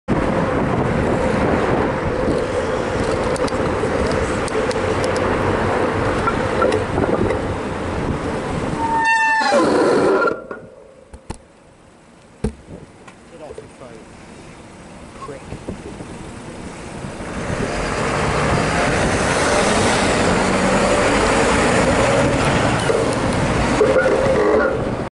Taxi Driver Phone (YF12 PKY)
Its great to get a cheery acknowledgement when you remind a driver he shouldn't be using his phone